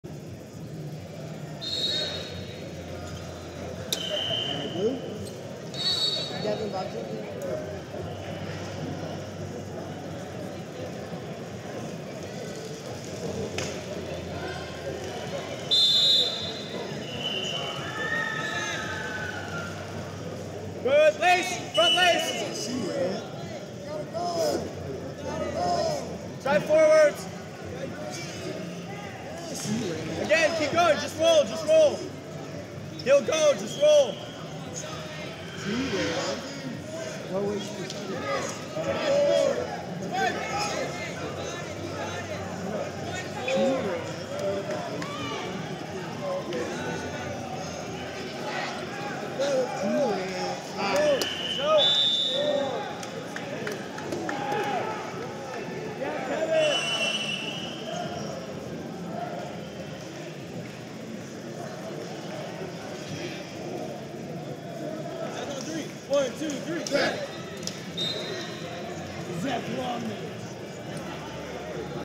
120 Lbs Quarters & 1st Wb (16 Team) - Devin Bobzien, California 1 Vs Charlie Irwin, North Dakota R
Flosports-partner-access/2023 Junior National Duals And 14U Girls Duals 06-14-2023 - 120 Lbs Quarters & 1st Wb (16 Team) - Devin Bobzien, California 1 Vs Charlie Irwin, North Dakota Red 4fda